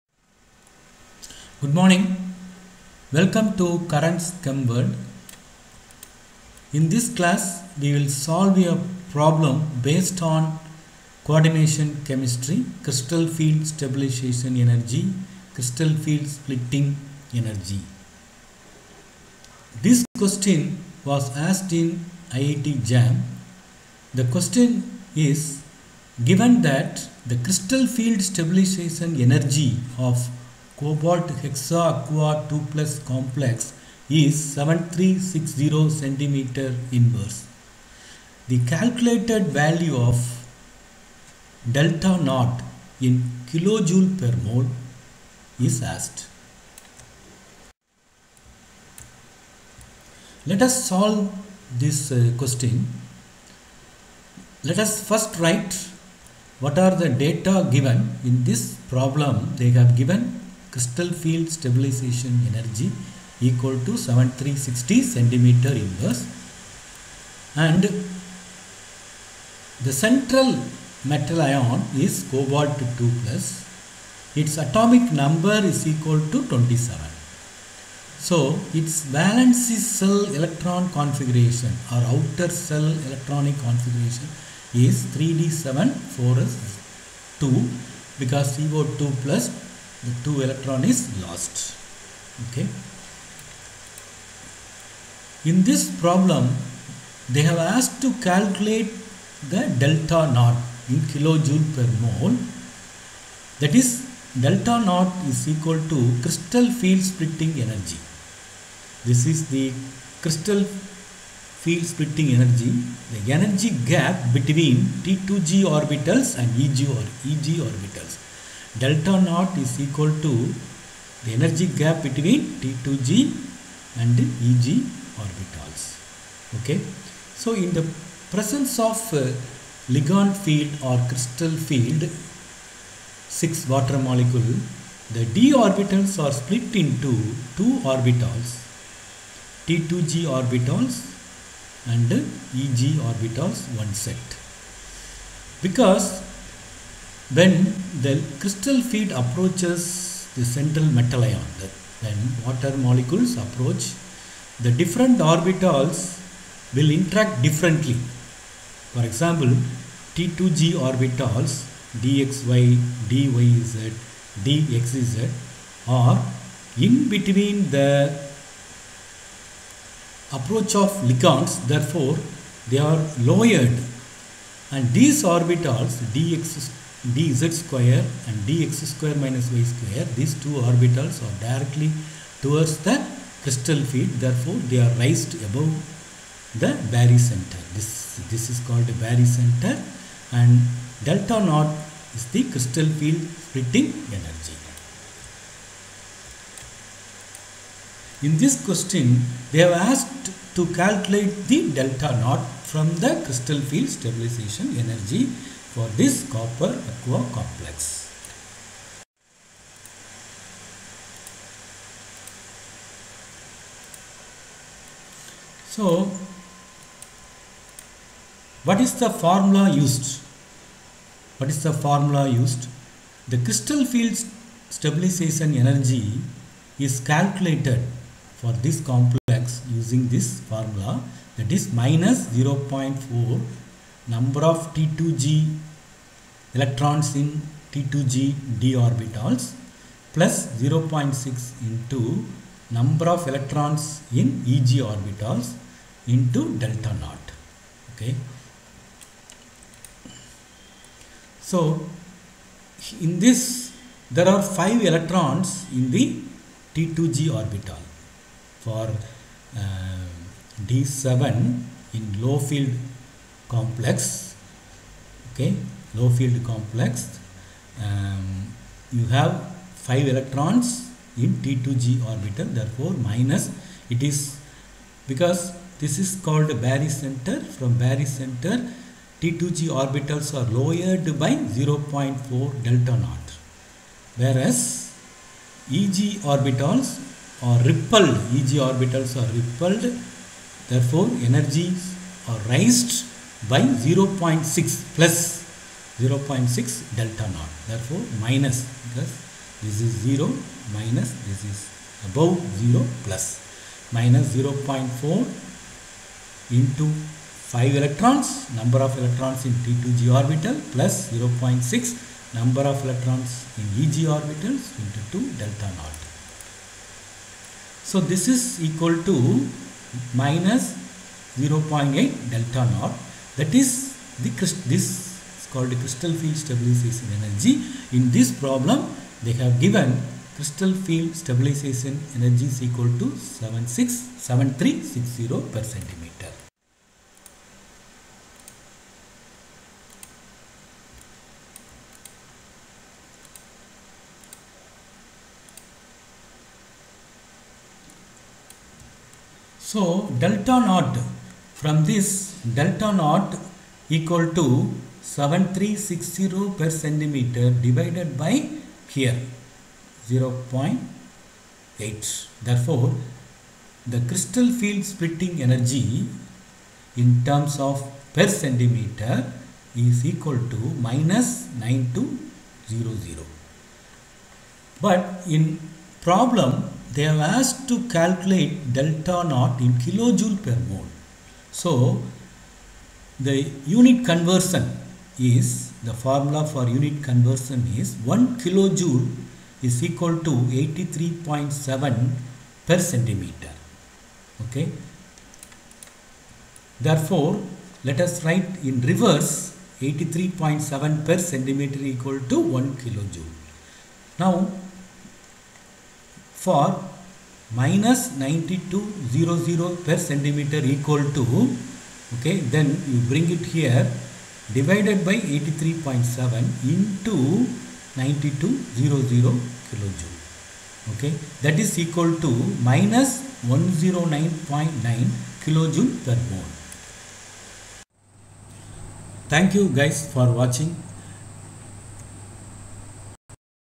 Given that the crystal stabilization CFSE of [Co(H2O)6]2+ is 7360 cm-1, Find delta o in kJ mol-1.
Given that the crystal stabilization energy (CFSE) of [Co(H2O)6]2+ is 7360 cm-1, the calculated value of delta o in kJ mol-1.
Crystal field splitting energy delta o
unit conversion  1 kJ = 83.7 cm-1
coordination chemistry
iit jee jam neet sat gate csir net ugc trb competitive exam upsc tnpsc